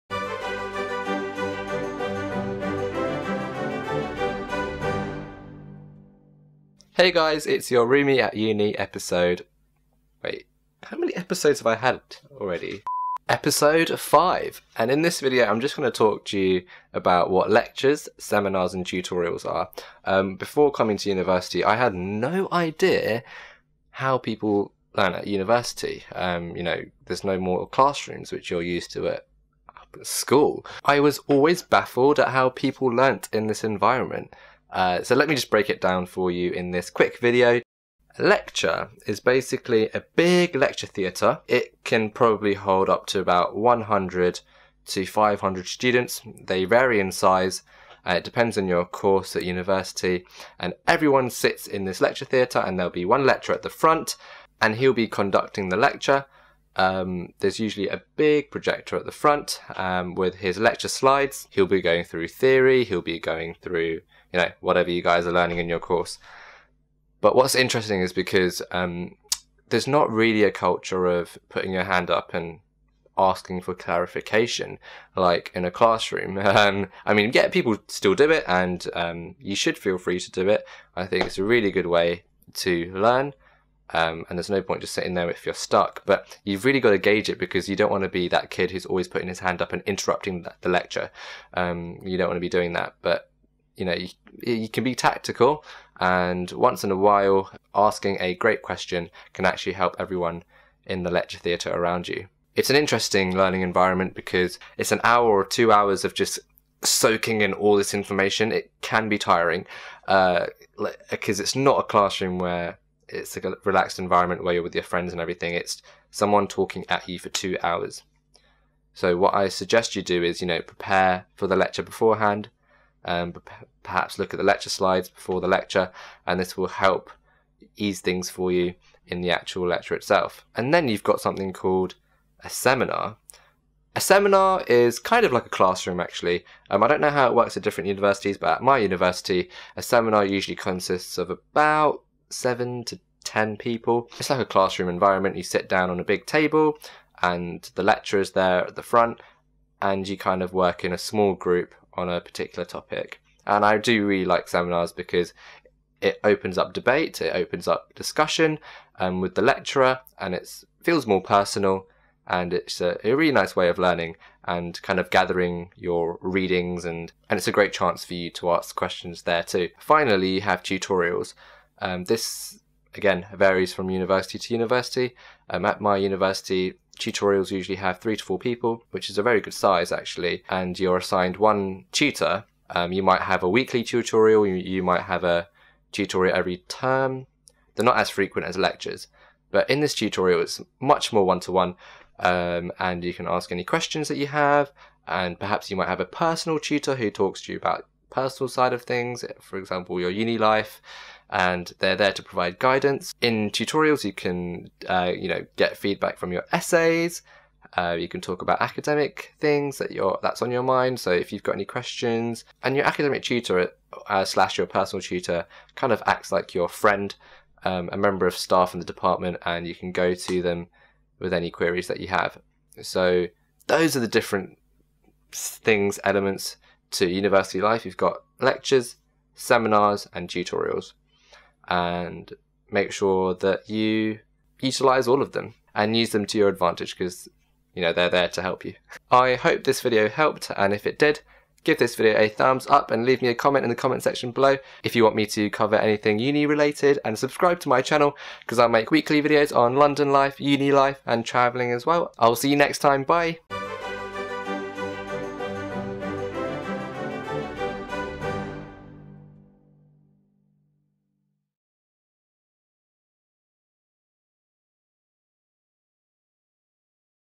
Uni ADVICE: What are Lectures, Seminars and Tutorials? (Ep.5)
Before starting university, I had no idea what lectures were. I was really confused because we were so used to the classroom learning environment. Watch this video to find out how we study at university!

Anpu